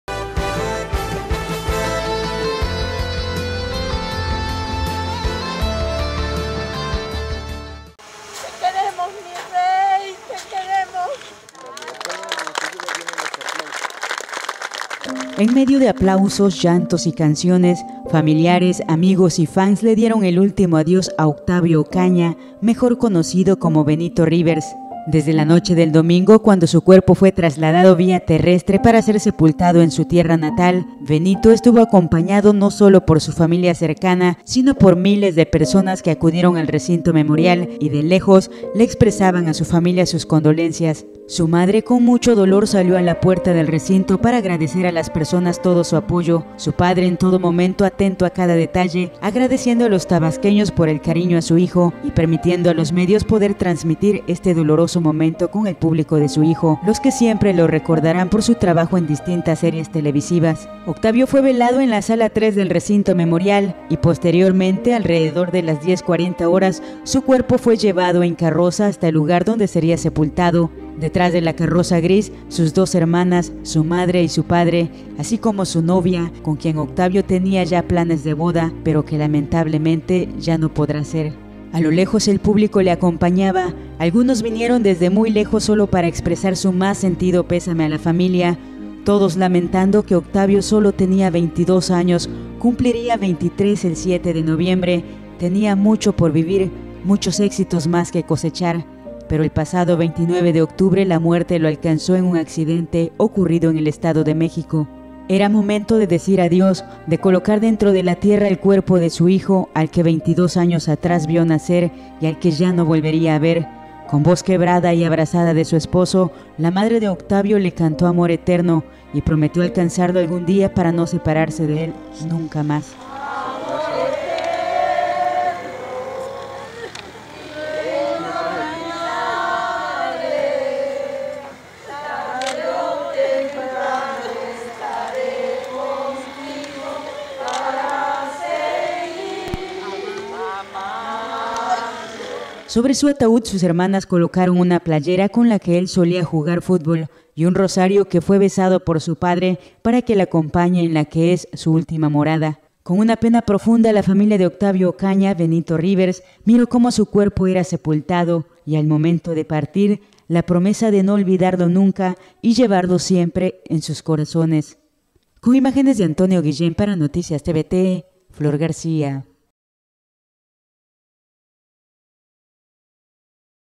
Con aplausos, llantos y canciones dan el último adiós a Octavio Ocaña
NoticiasTVT🟣 En medio de aplausos, llantos y canciones, amigos y familiares dieron este lunes el último adiós a Octavio Ocaña, en el Recinto Memorial. Sus papás agradecieron a tabasqueños las muestras de cariño a 'Benito Rivers'.